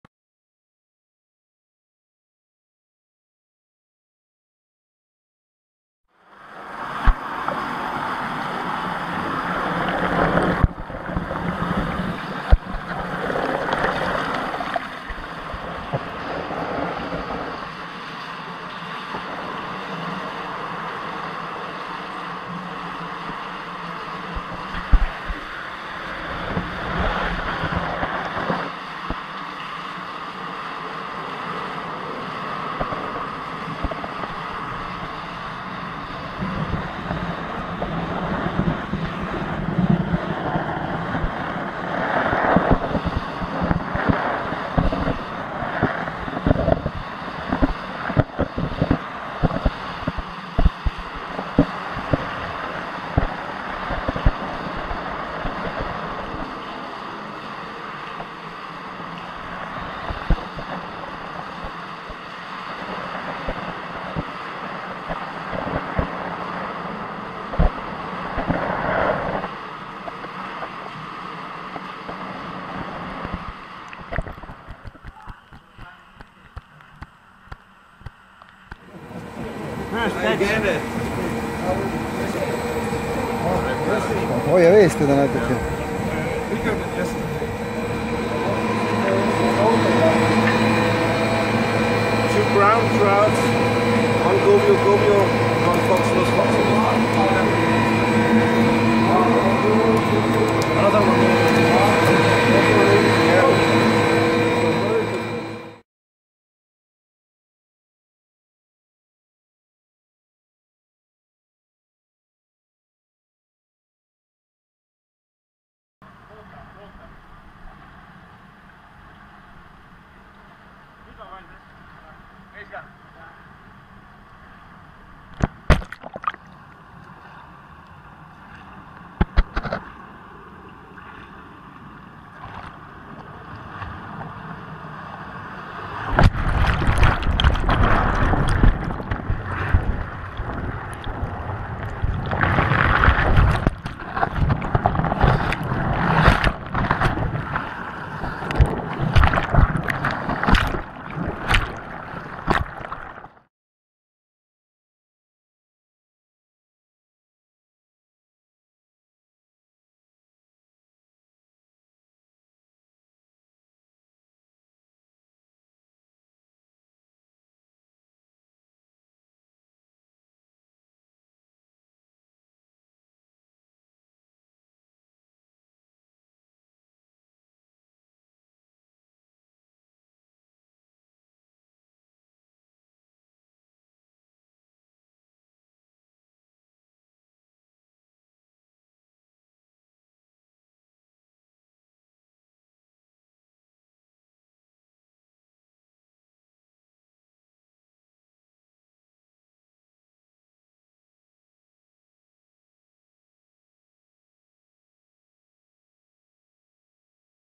Fish route through archimedian screw
All the material was filmed in Austria Pilsing. Camera was placed in the archimedian screw to film fish path up and down stream. Filming took place in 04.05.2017.